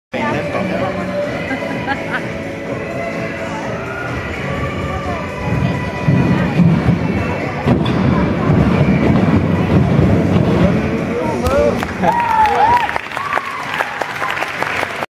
2012 British Open Abdullah TUMBLING Prelims 1st pass
Here is Abdullah competing in the Senior Men's Tumbling category at the 2012 British Open Tournament for Acrobatic Gymnastics and Tumbling.